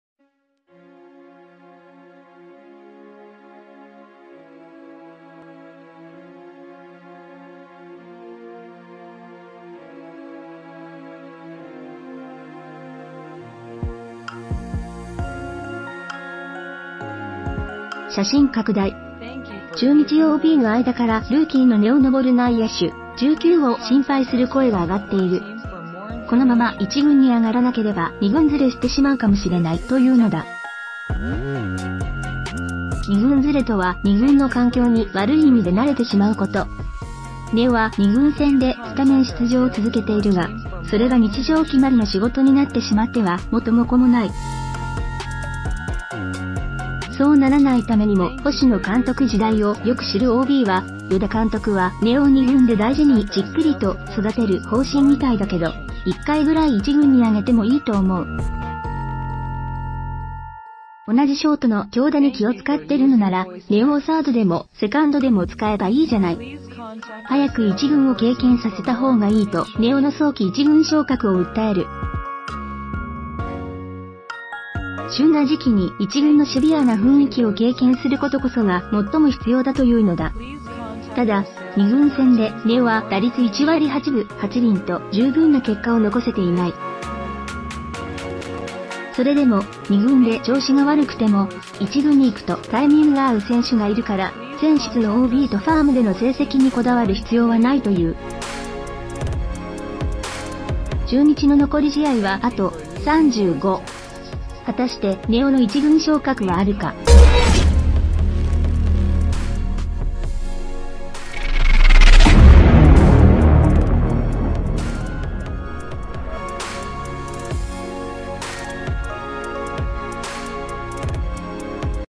中日・根尾 心配される“二軍ズレ”Top News
,News,中日・根尾　心配される“二軍ズレ”Top
中日・根尾 心配される“二軍ズレ”Top News ► Subscribe for more 写真拡大 中日ＯＢの間からルーキーの根尾昂内野手（１９）を心配する声が上がっている。「このまま一軍に上がらなければ二軍ズレしてしまうかもしれない」というのだ。「二軍ズレ」とは二軍の環境に悪い意味で